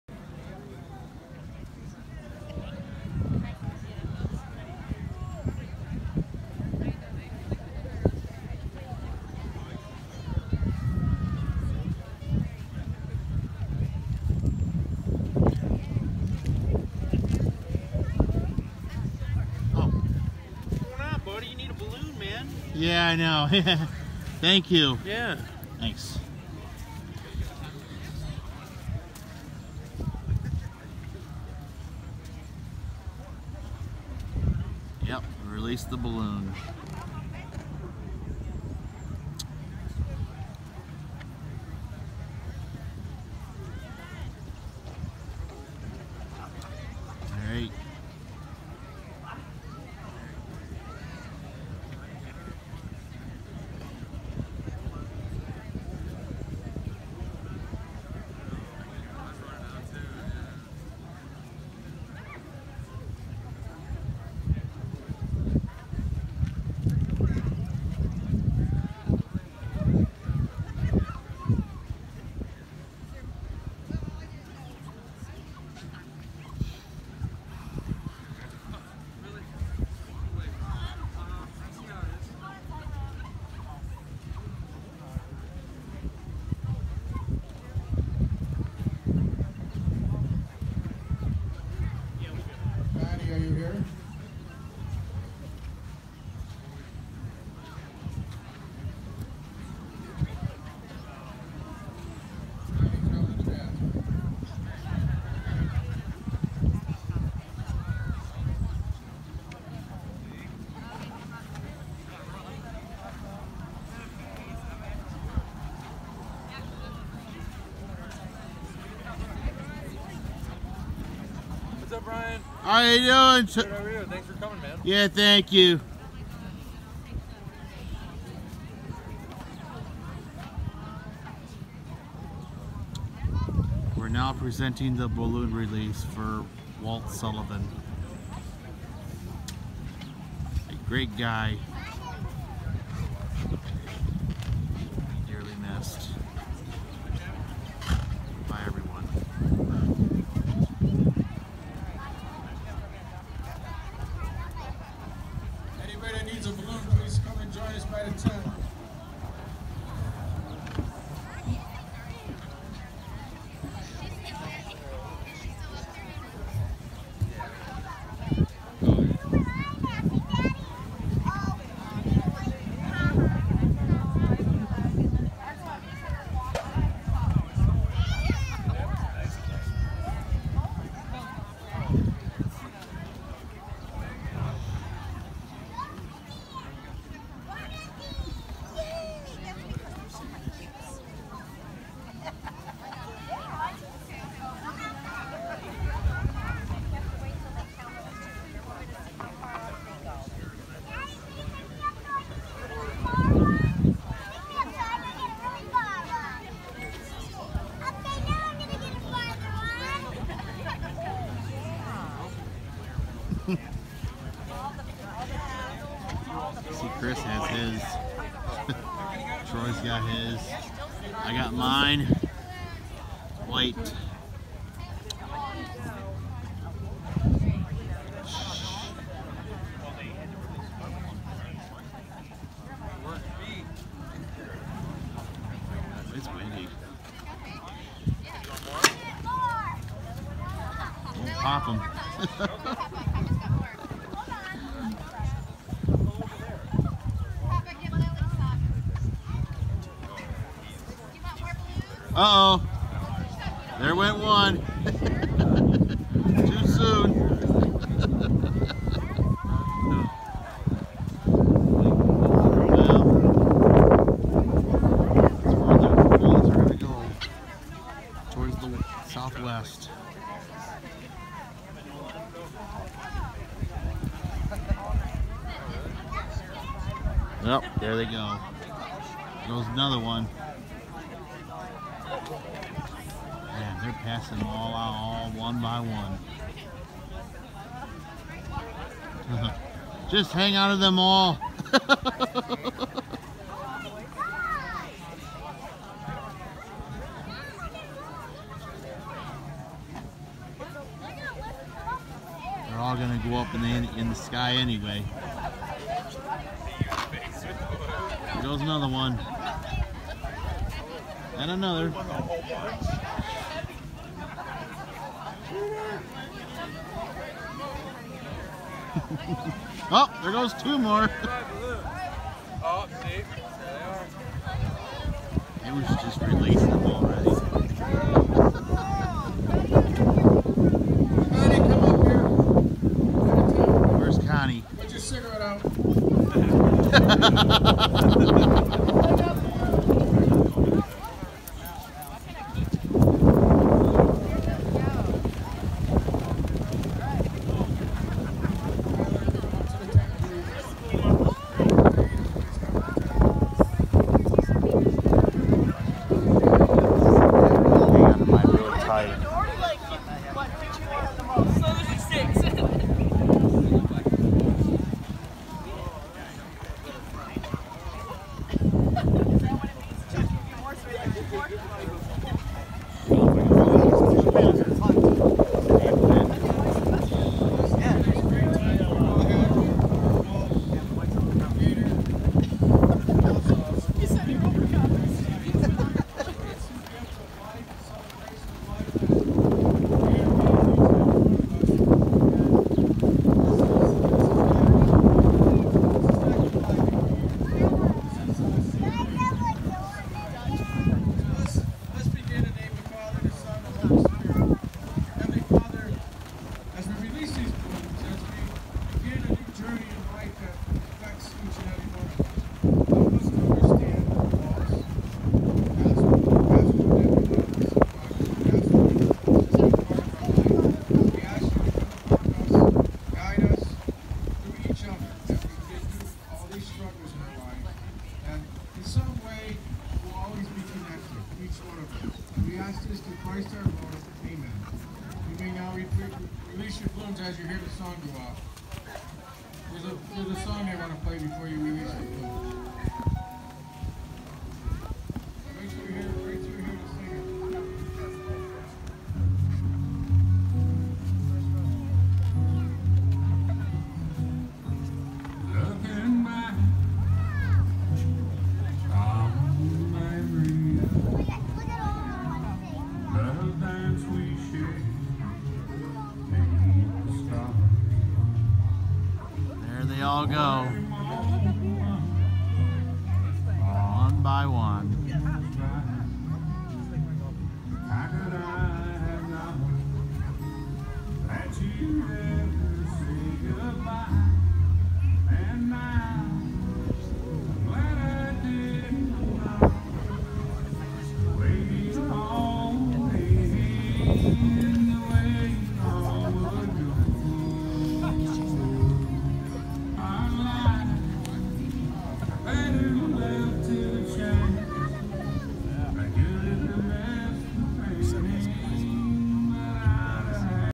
The Celebration of life for Walt Sullivan
Family and friends gather to celebrate the life of Walt Sullivan as they release balloons up into the sky at Round Up Lake Campground in Mantua, Ohio.